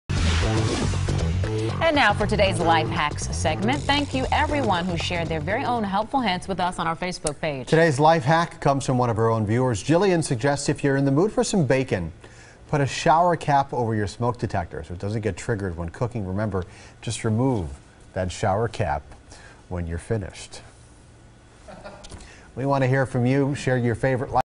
Viewer's Choice Life Hack: smoke alarm trick
If you're in the mood for some bacon, put a shower cap over your smoke detector so it doesn't get triggered when cooking.  Just remeber to remove the cap when finished.